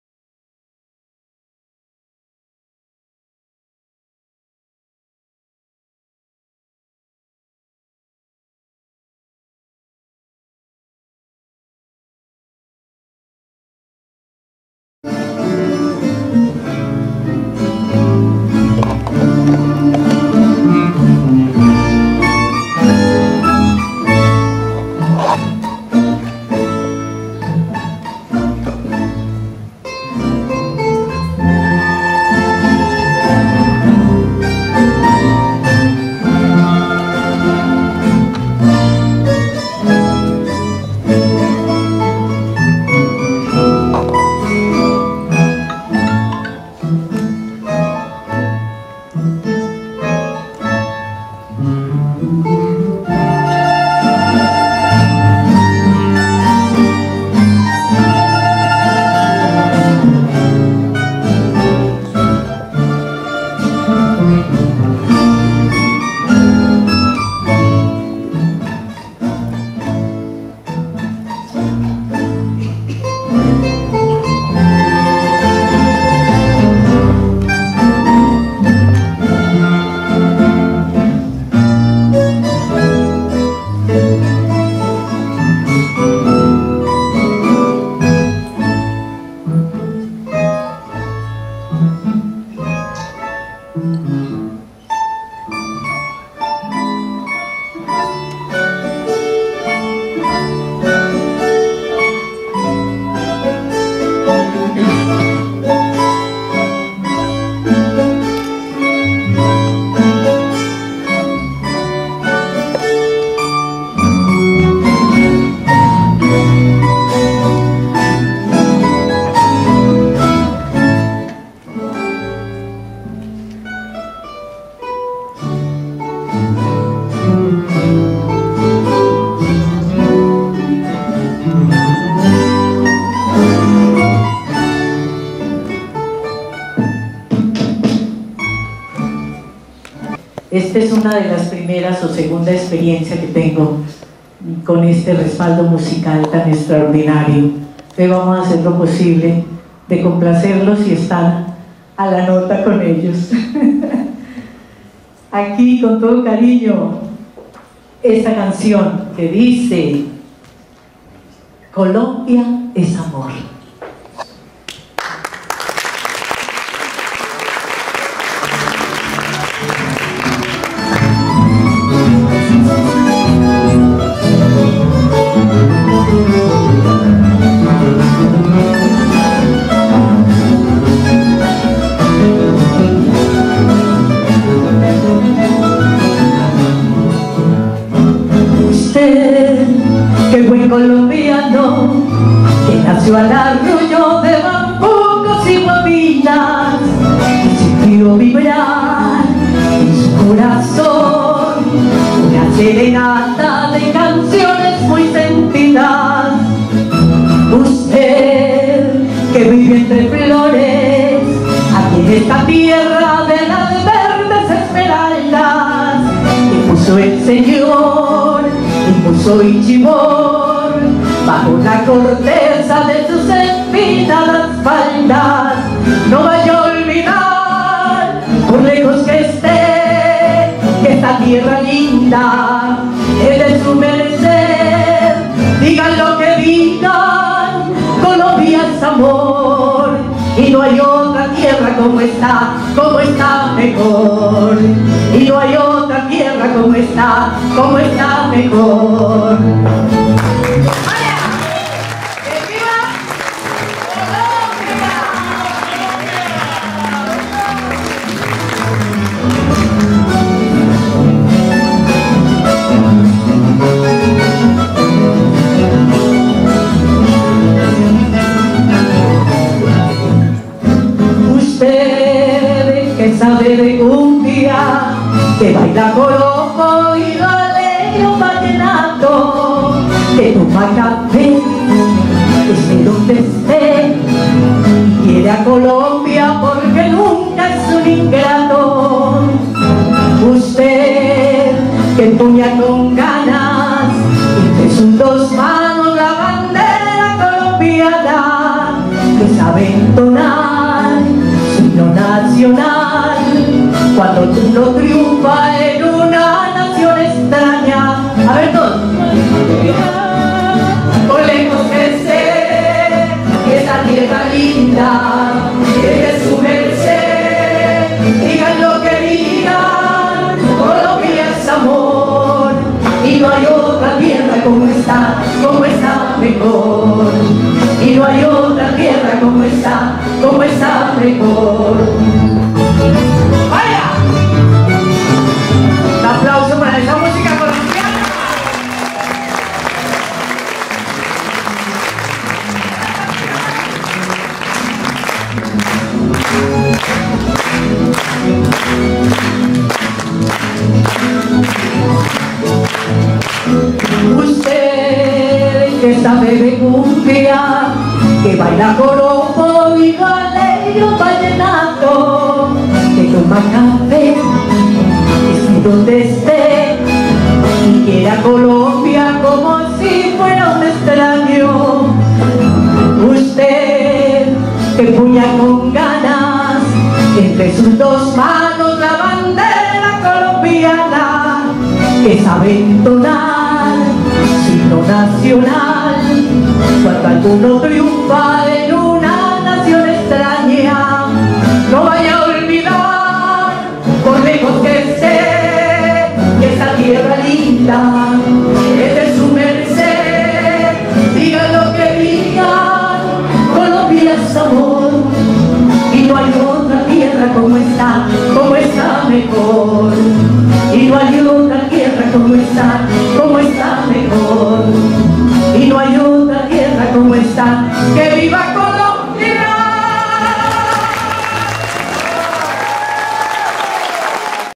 Estudiantina Colombia en concierto, homenaje a Rafael Mayorga, II parte, Bogotá, 2024
Concierto en homenaje en vida al maestro Rafael Mayorga, por parte de la Estudiantina Colombia, evento realizado el 26 de septiembre del año 2024 en el auditorio de la Universidad Santo Tomás. Durante 62 años, el maestro Rafael Mayorga Rodríguez, quien fue cantante de la Estudiantina Colombia, participaron como invitados, Marinella y José  Luis Ortiz, como maestro de ceremonia, estuvo el comunicador social Guillermo Bohorquez.